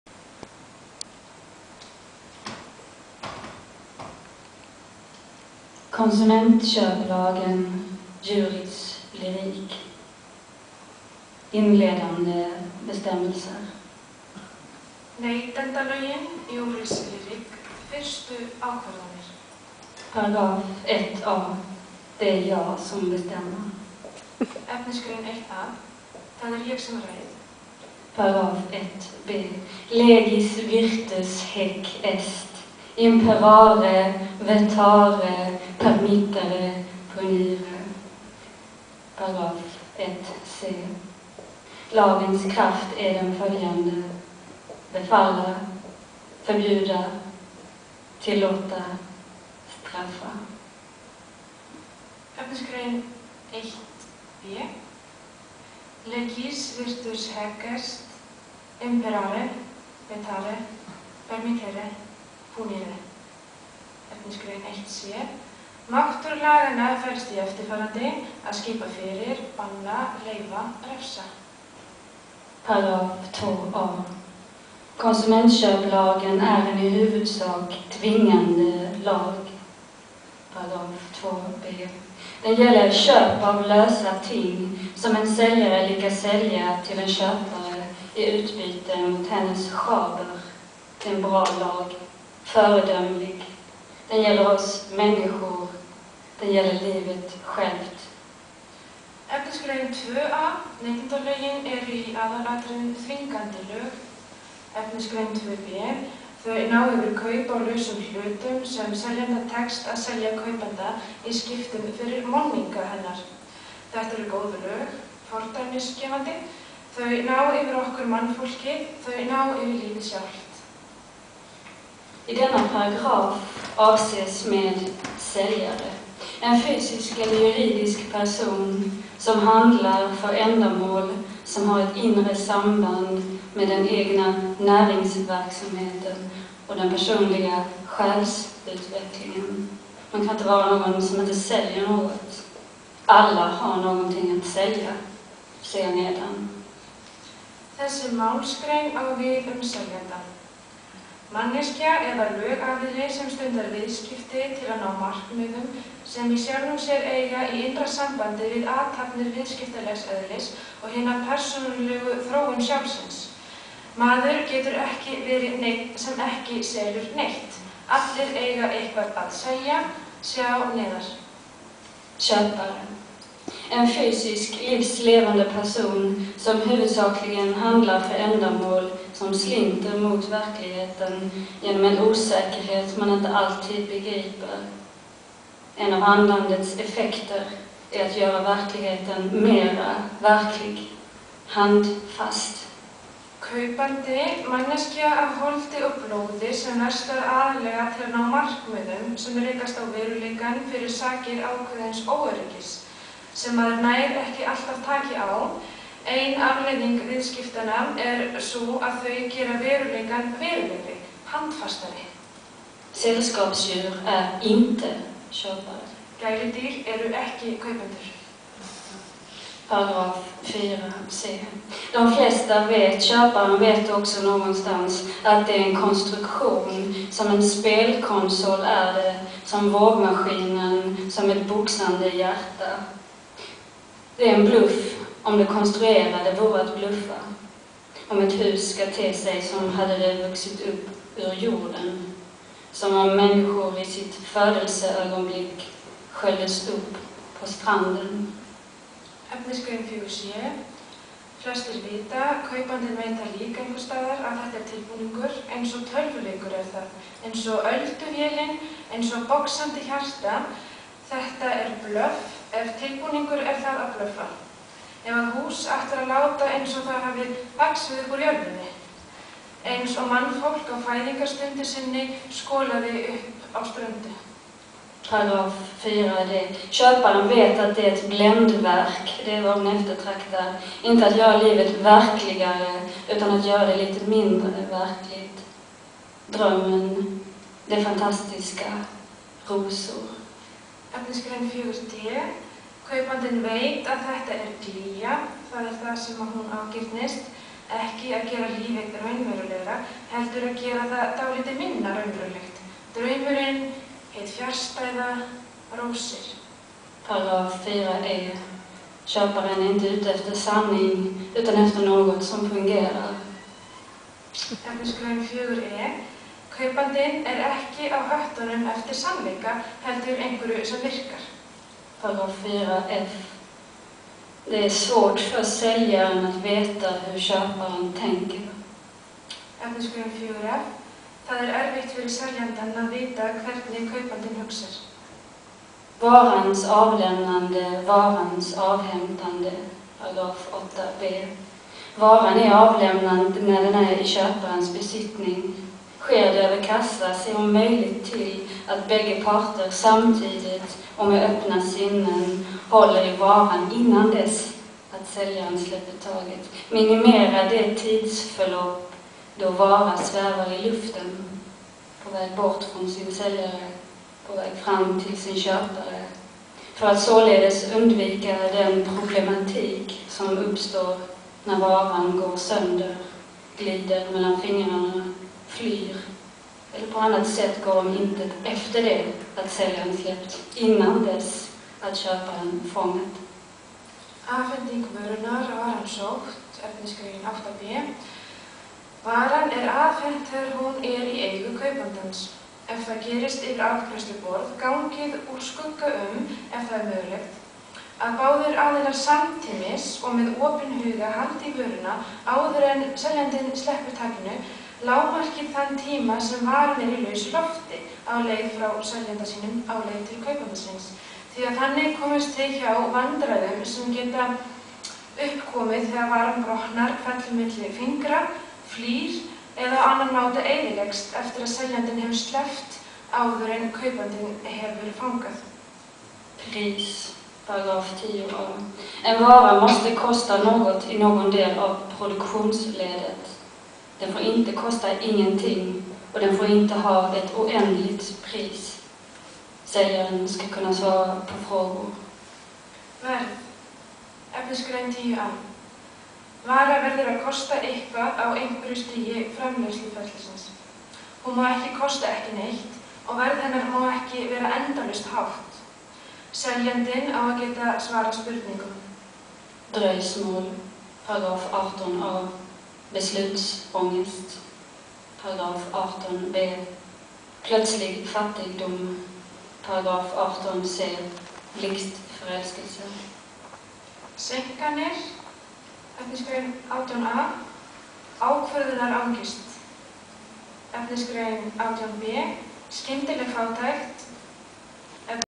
Ida Börjel á Nýhilhátíðinni
Ida Börjel les úr Konsumentköplagen (Neytendalögin) á ljóðahátíð Nýhils 2008. Bryndís Björgvinsdóttir les þýðingu Eiríks Arnar Norðdahl.